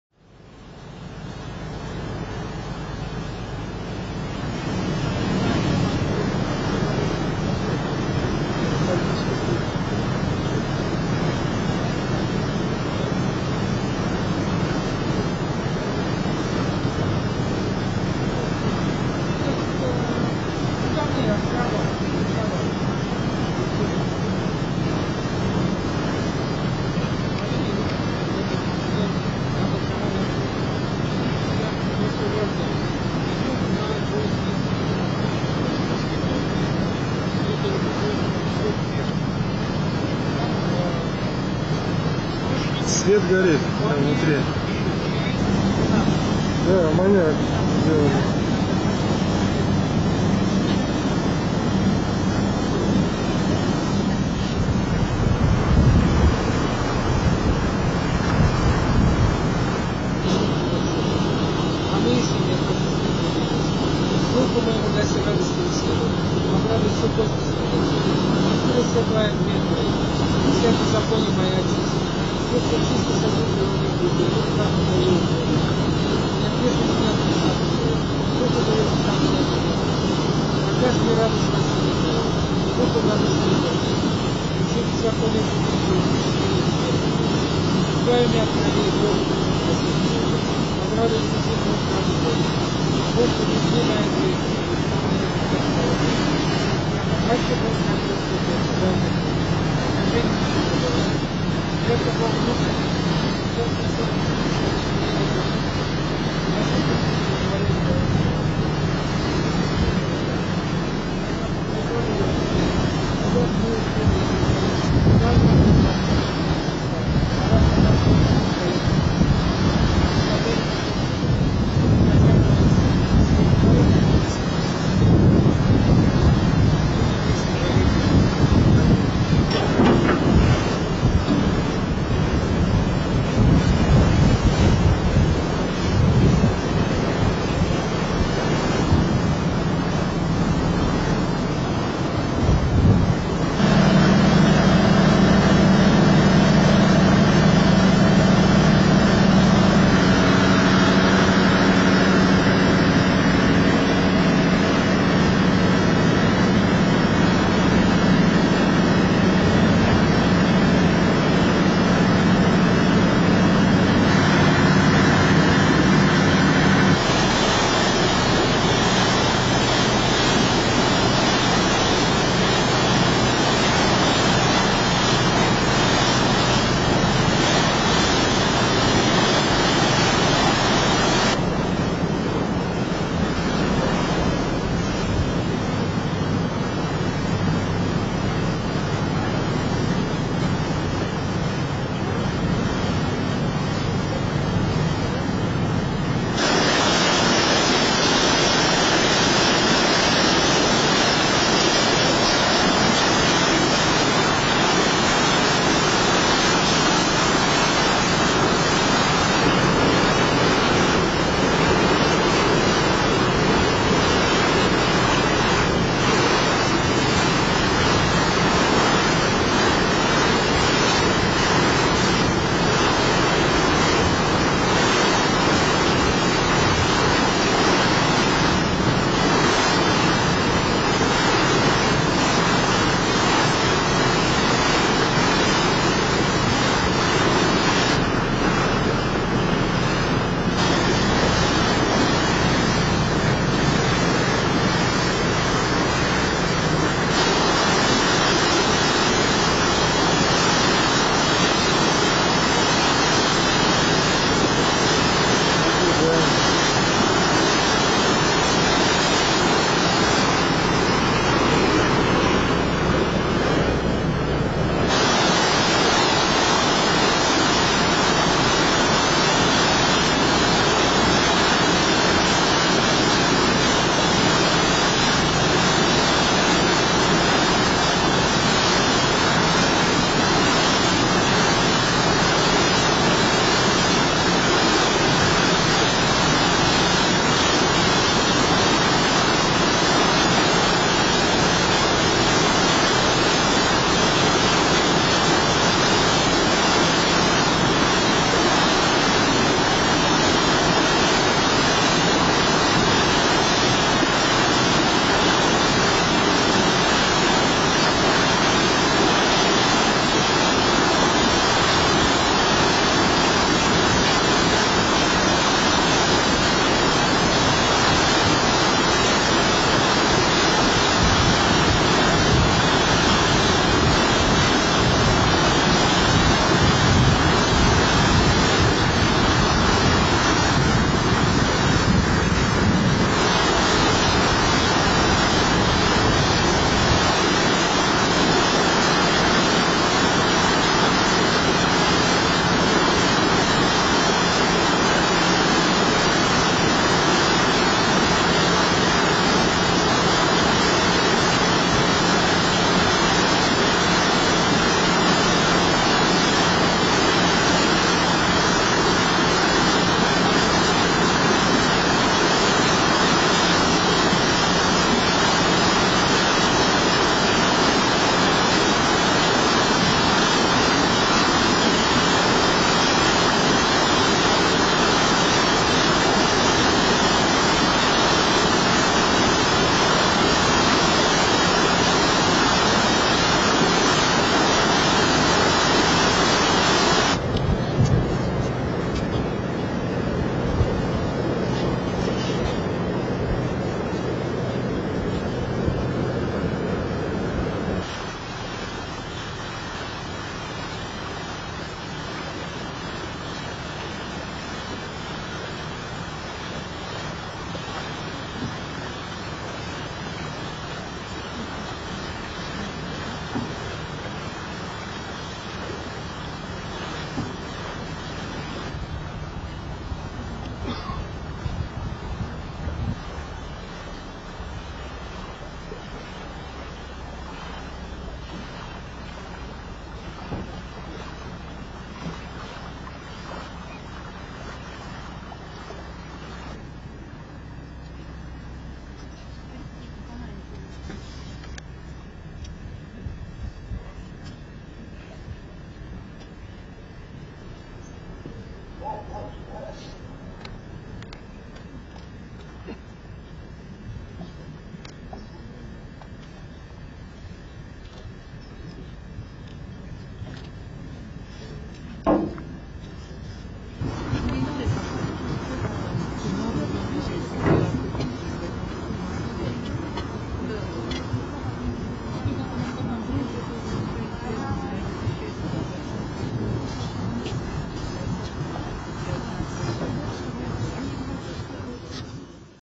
Соловки. Паломничество. ч.1 Прибытие
Паломническая поездка в Соловецкий монастырь в августе 2001г. Часть1. Прибытие нашей группы на Соловецкие острава.